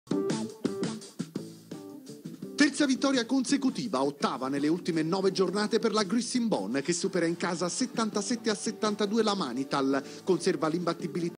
OJ baseline dunk vs torino
Grissin Bon Reggio Emilia's Ojars Silins (SF, 1993) dunk vs Manital Torino (17/01/2016)